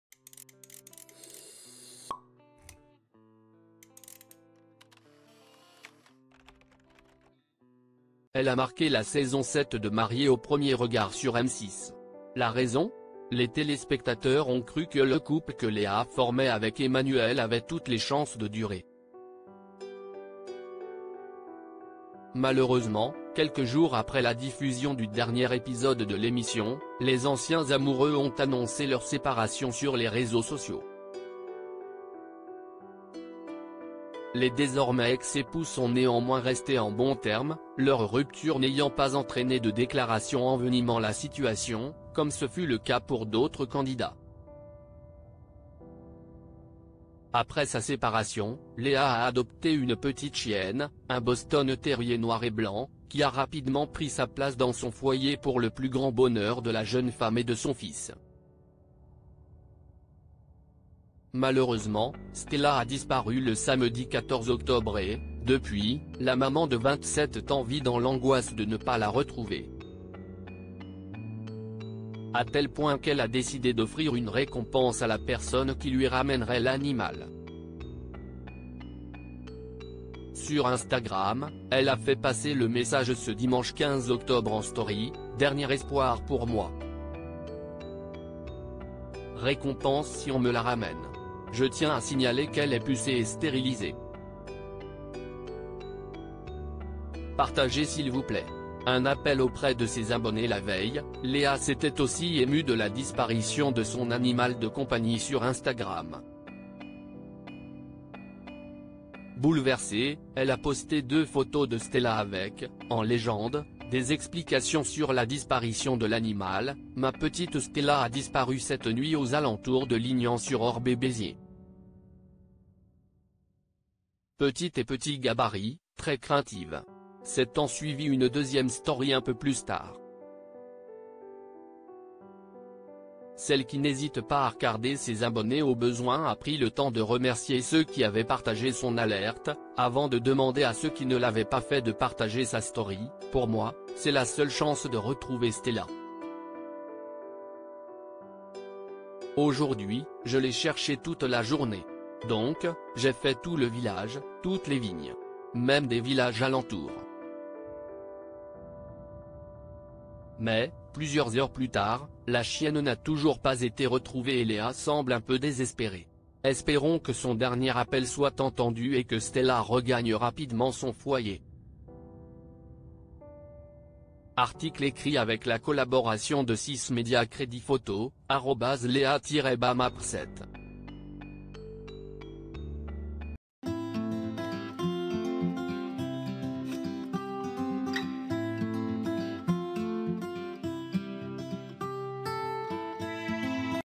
Léa Mariés au premier regard désemparée elle offre une récompense à celui qui retrouvera son chi
Léa (Mariés au premier regard) désemparée : elle offre une récompense à celui qui retrouvera son chien